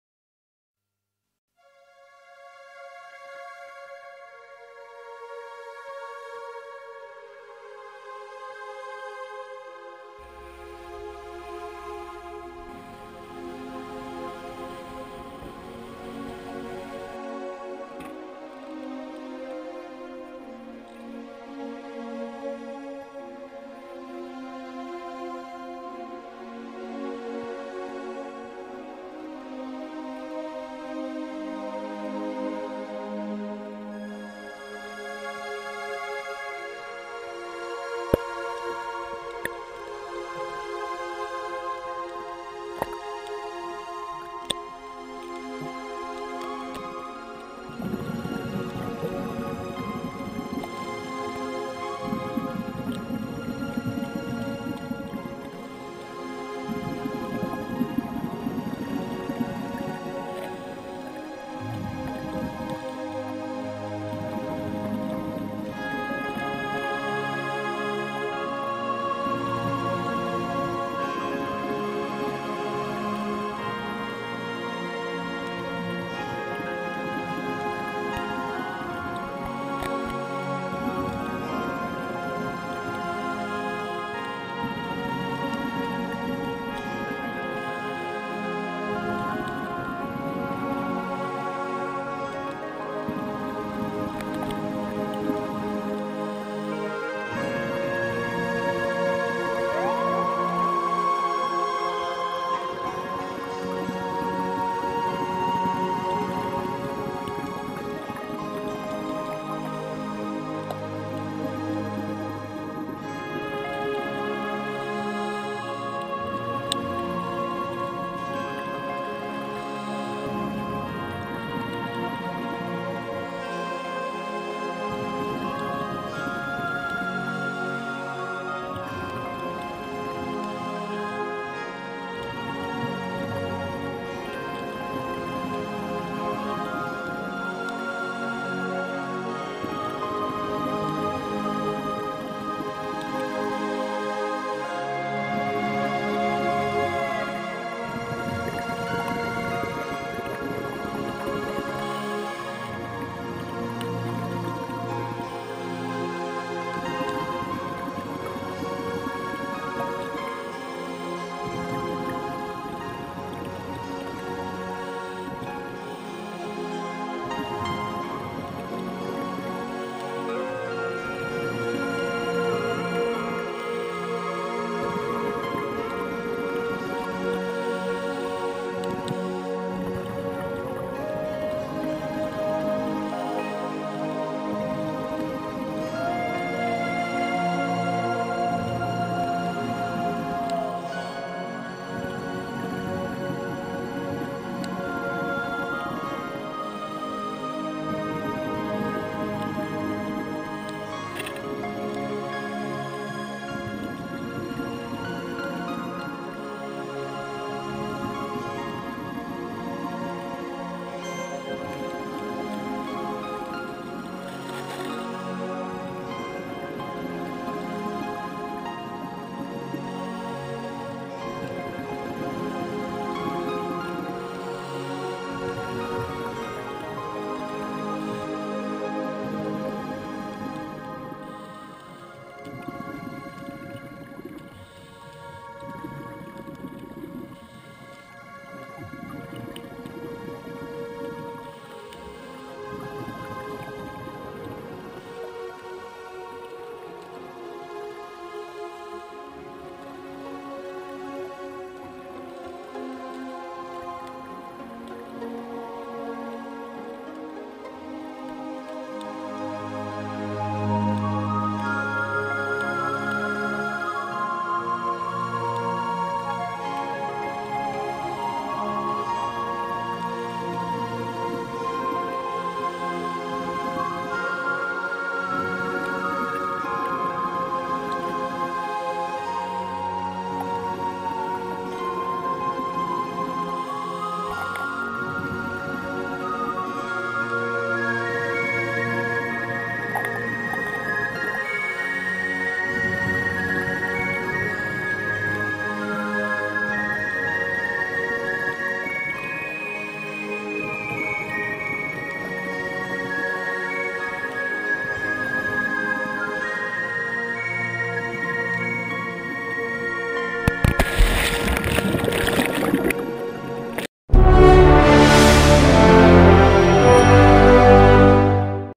Adventure diving near the fortress of Ile Sainte Marguerite
This was an awesome dive with AQUA EVOLUTION ) thanks for two good and exiting dives! it is not the last time I will dive with you.